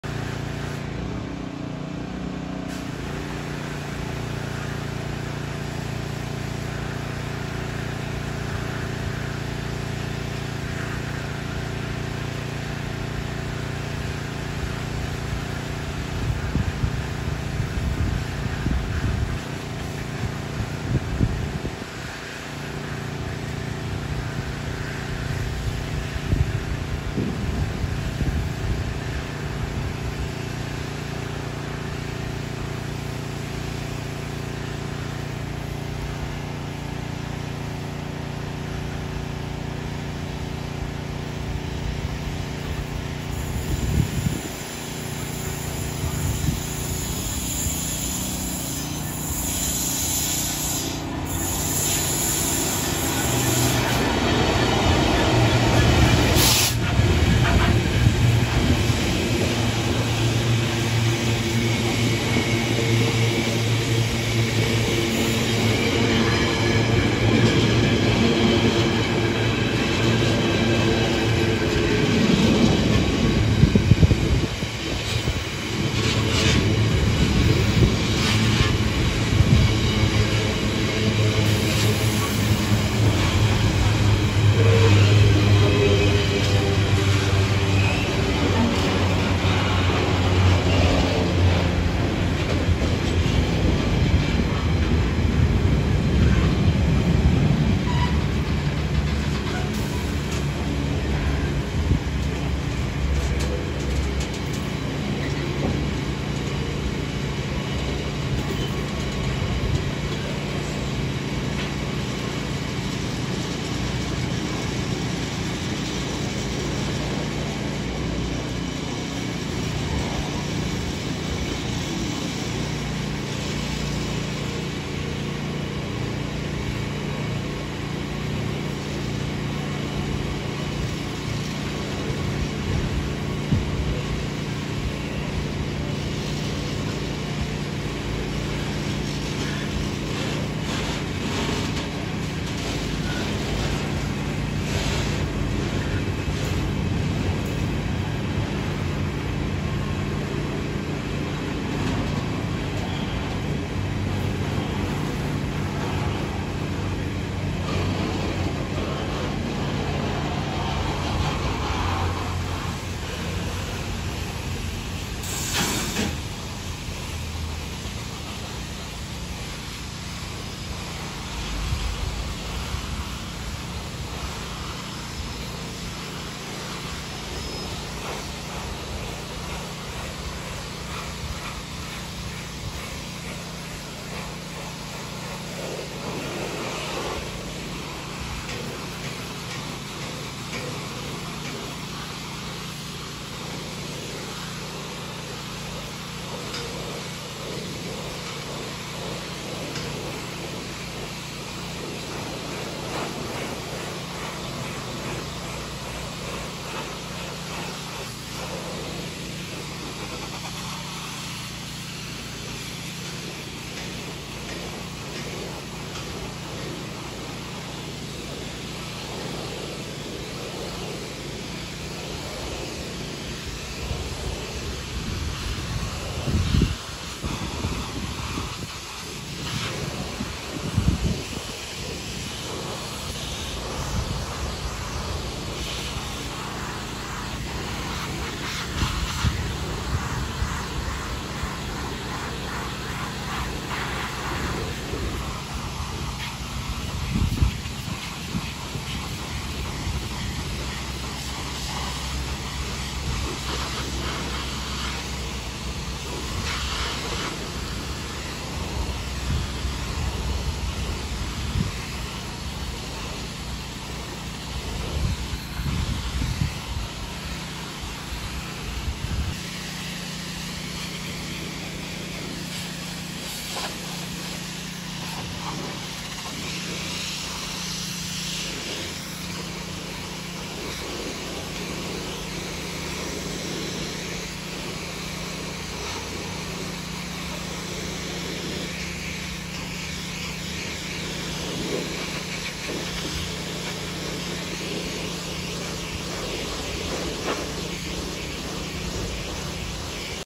“Golden Jubilee” 66705 GBRf 4L11 containers 12:49 6E Lea Road 25/9/2024
Still with petrol powered pressure washer noise! “Golden Jubilee” 66705 GBRf 4L11 Masborough N&W Gbrf to Felixstowe North Gbrf with 39 containers 12:49 running 6E Lea Road Gainsborough 25/9/2024

Last train;

Next train;